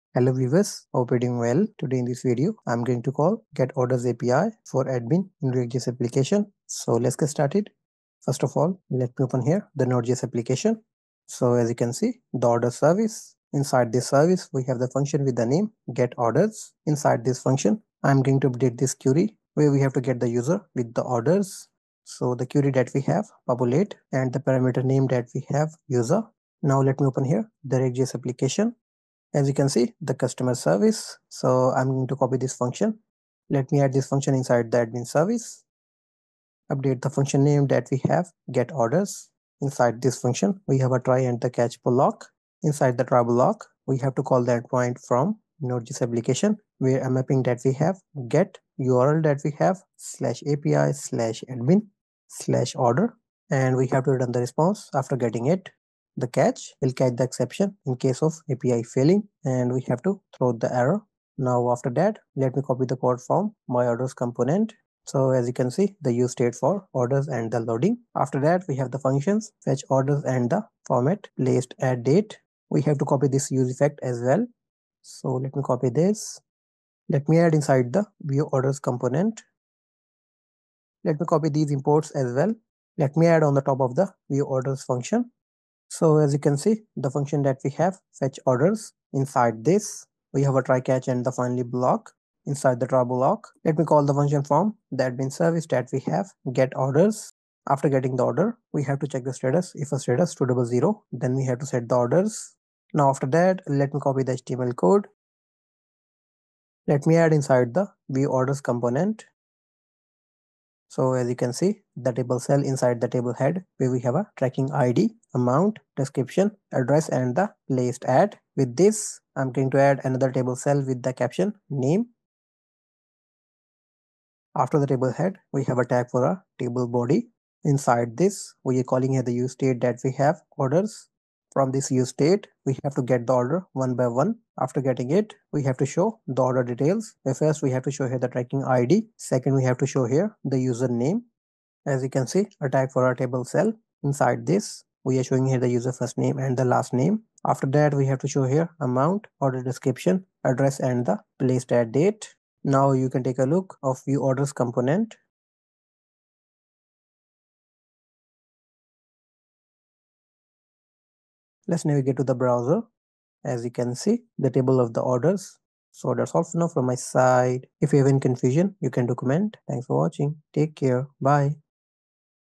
Fetch & Display Placed Orders in React JS Application | Book Store Project Using MERN Stack | #42
In this video, you will learn:
✅ How to call the Get Placed Orders API for the admin in a React JS application.
✅ Fetch and display customer orders on the admin orders page.
✅ Implement API integration to manage order data dynamically.
✅ Best practices for handling and rendering orders efficiently.

By the end of this tutorial, you'll have a functional Orders Page where the admin can view all placed orders.